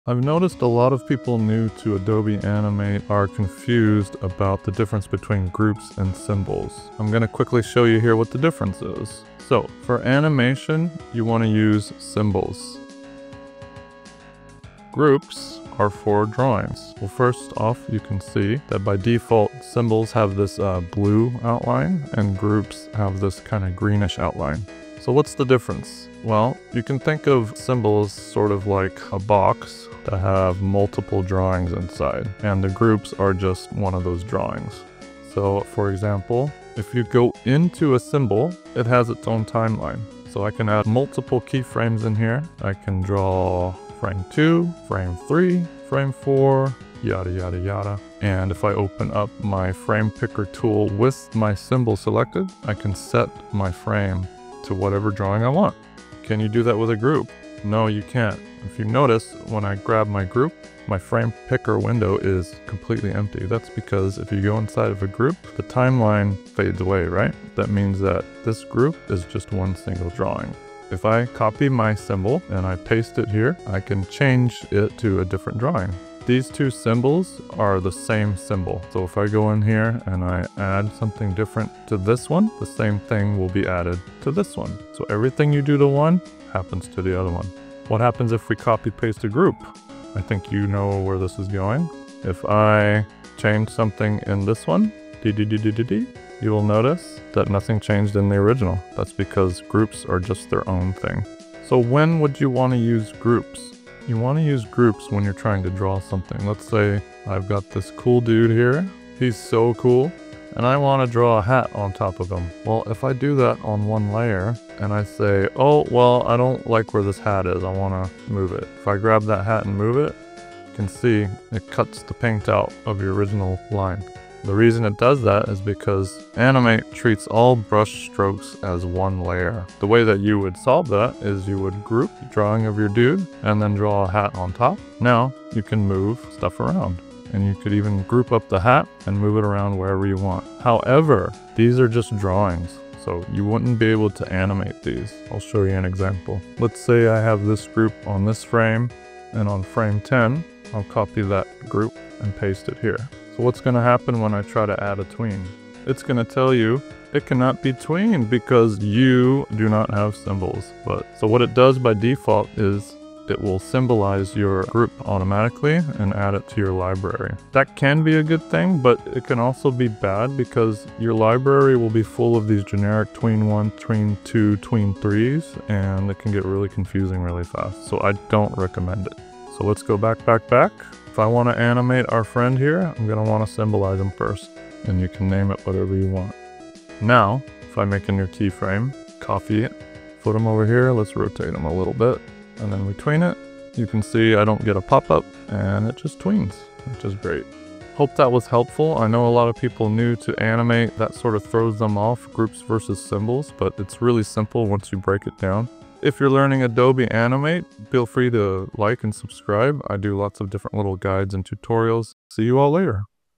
Groups vs Symbols - What's the difference? [Adobe Animate 2025]
In this vid I do a quick run-down of the difference between symbols and groups in Adobe Animate. I noticed a lot of people new the the software get confused by this & I didn't see any recent tutorials for this for 2025. Enjoy!